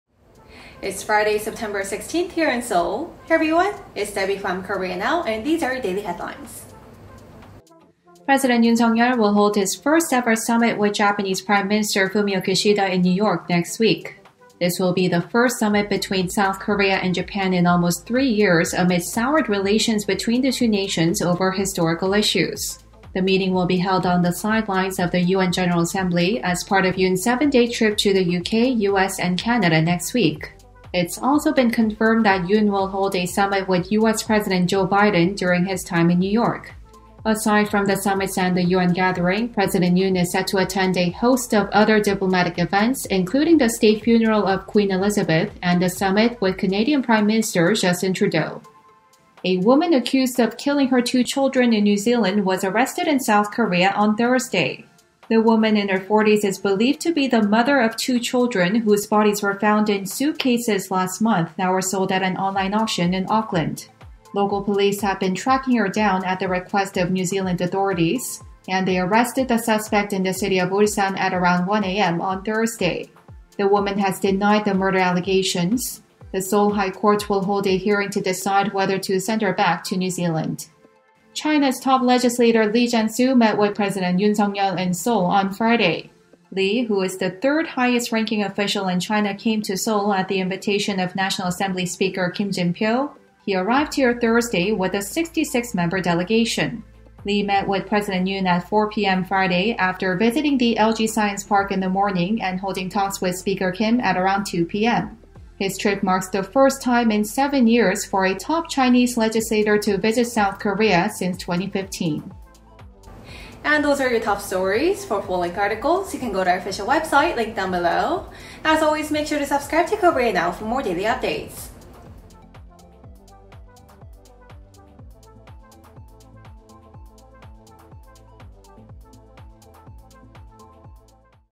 Yoon to hold first summit with Japanese PM / China’s No. 3 official meets President Yoon in Seoul
1. Yoon to hold first summit with Japanese PM next week  00:07

2. New Zealand suitcase murders suspect arrested in S. Korea  00:49

3. China’s No. 3 official meets Pres. Yoon in Seoul  01:21